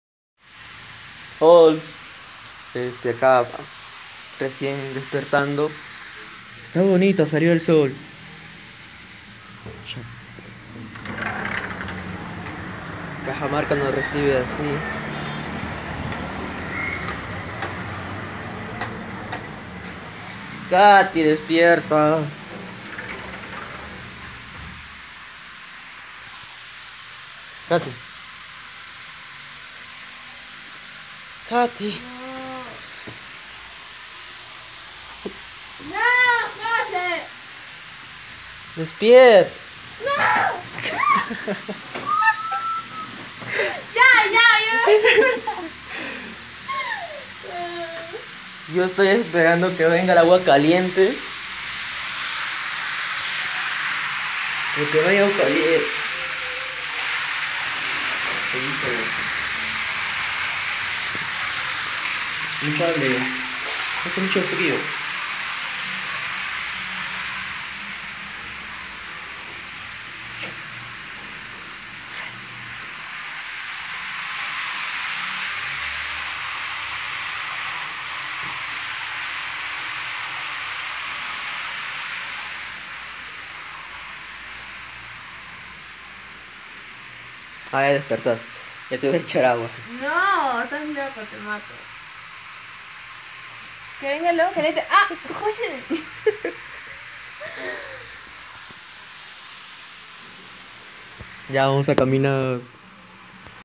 Me fui de viaje con mi mejor amiga!
Me fui a Cajamarca-Perú con mi mejor amiga